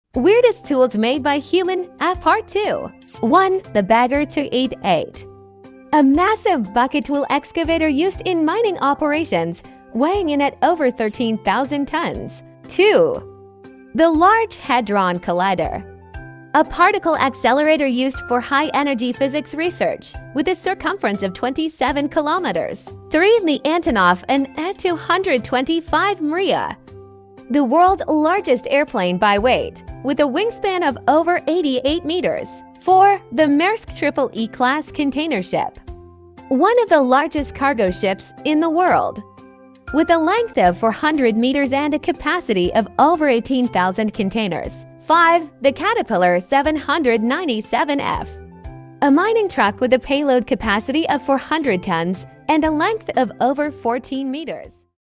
Weirdest Tools Made By Human | Part 2 | Unknown#6
✌️ Welcome to our video about the weirdest things ever created by humans!